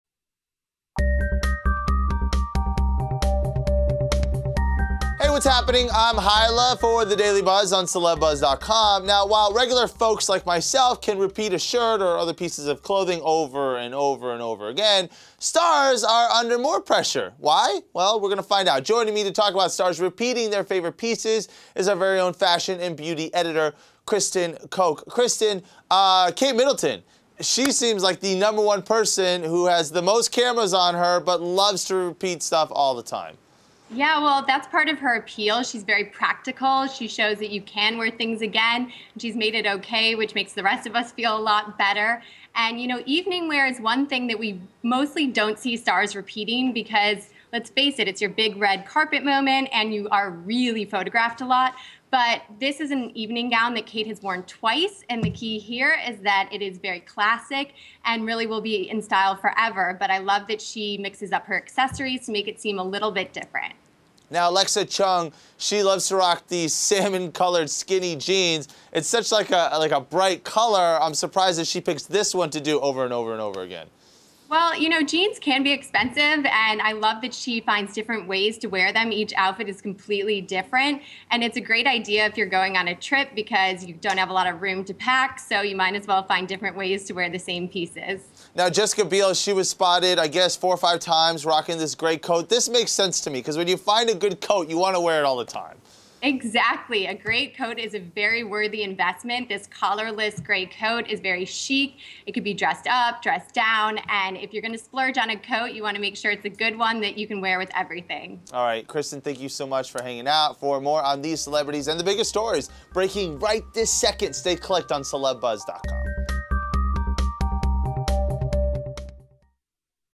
Kate Middleton & Other Star Fashion Repeaters
Kristin Koch joins to talk about stars wearing the same pieces over and over again.